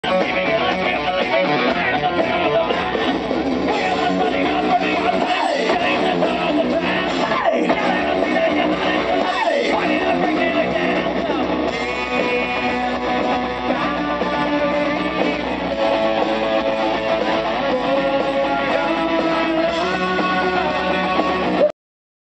Hail the Villain @ Uproar Atlanta
Really short video of Hail the Villain playing @ the Atlanta Uproar Fest.